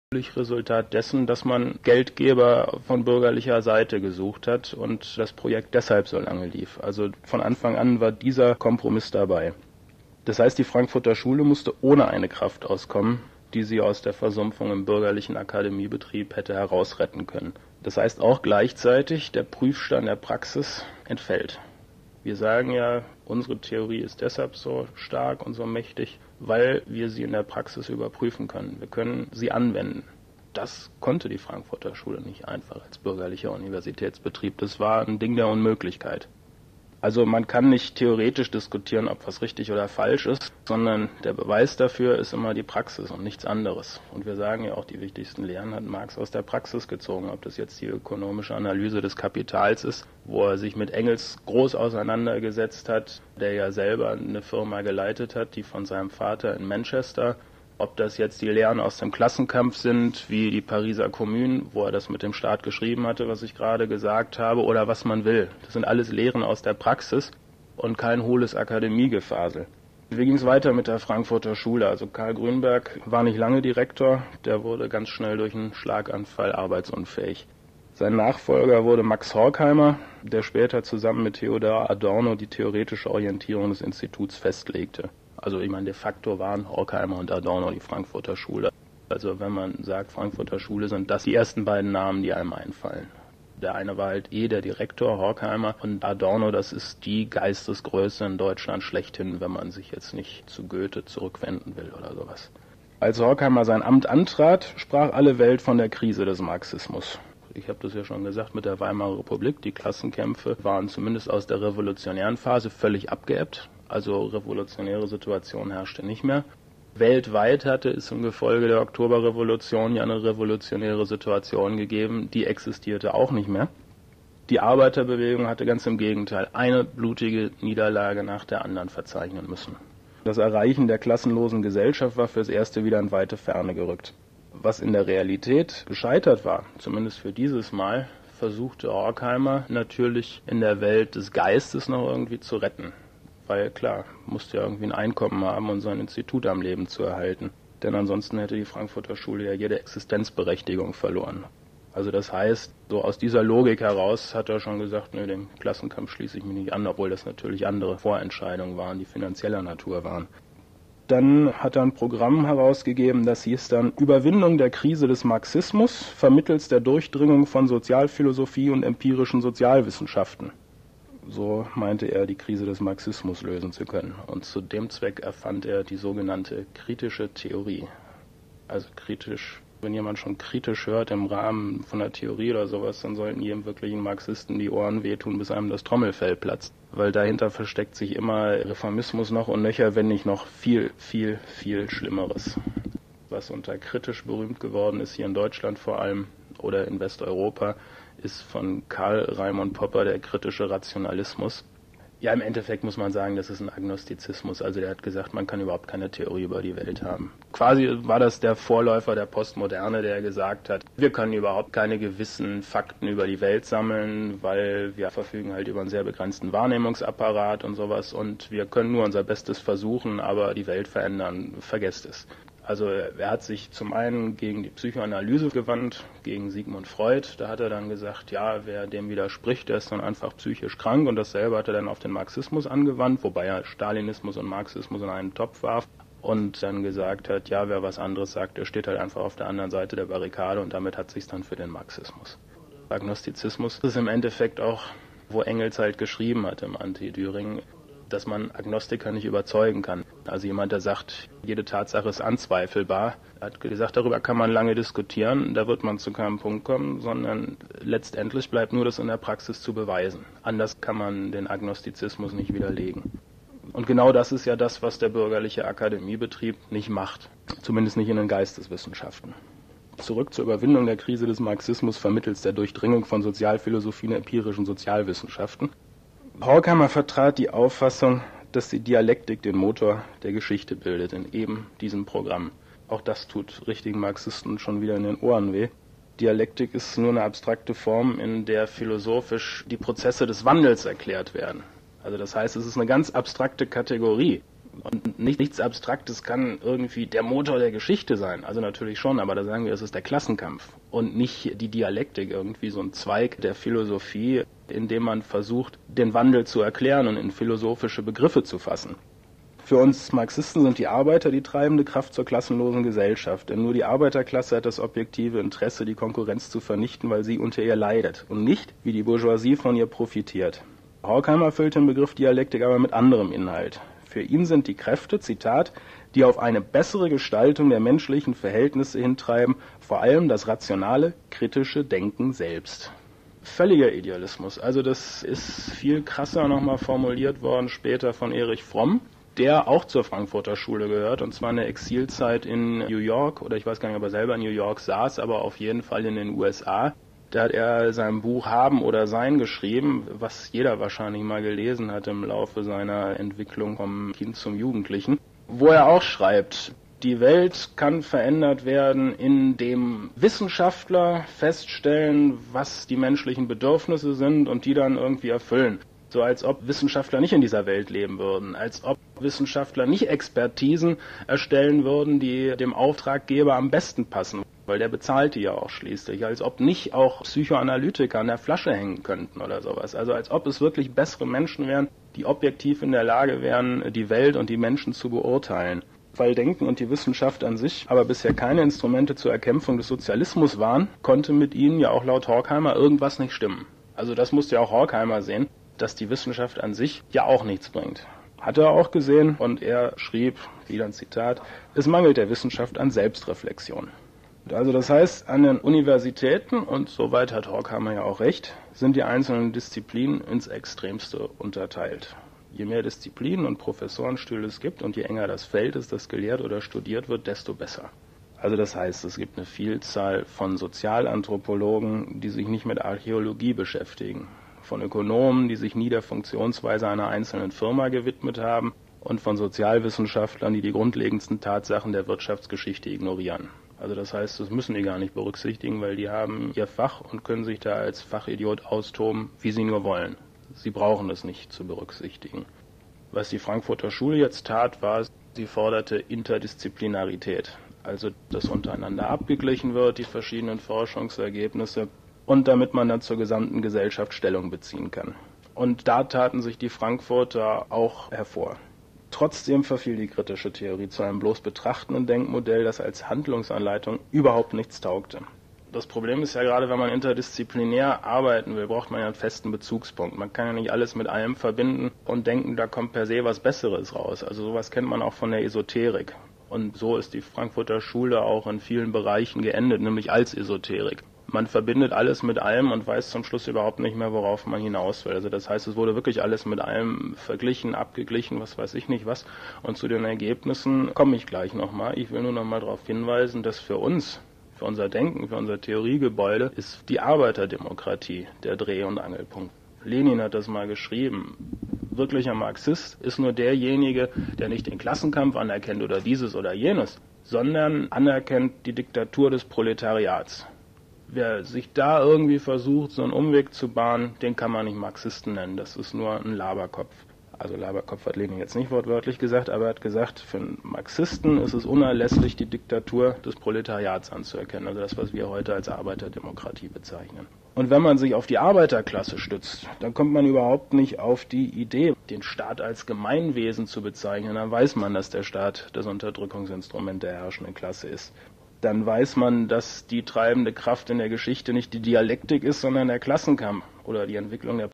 Frankfurter Schule (2/4) - Internationale Sozialisten
Referent: Carsten Schmidt

Aufnahmedatum: 2001

Weder Washington noch Moskau, sondern für internationalen Sozialismus